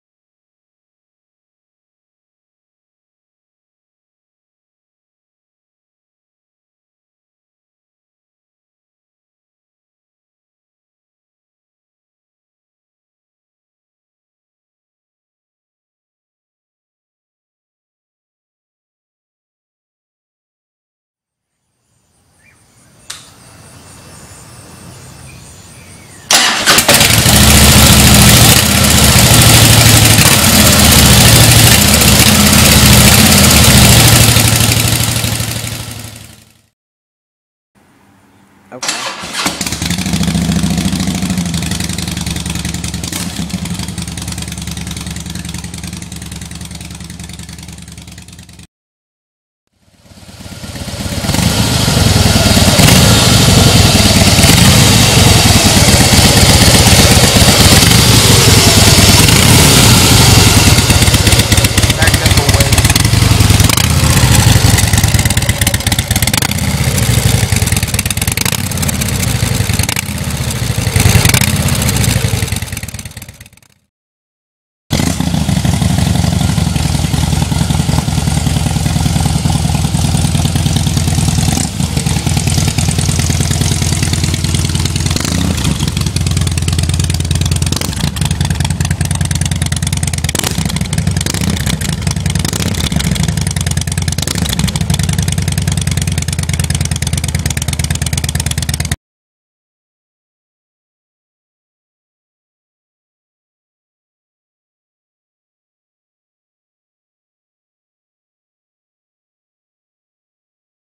Rush 1.75" Upgrade
I was looking for a better Harley sound than what my stock pipes offered. I didn't want anything "too loud", out of respect for my elderly neighbors. I also didn't want to wreck my budget. After consulting my Black Diamond H-D parts department go to guy, he recommended a pair of Rush slip-on mufflers with 1.75 inch baffles. I am very impressed and pleased with the difference. Sorry, I don't have the best video recording device (shot this with my iPod. Video's audio really don't do the difference justice. If you are looking to upgrade and are on a budget like I was, stop by your local H-D and chances are, they will have your stock model bike on the floor with the upgrade I have here. Give it a listen.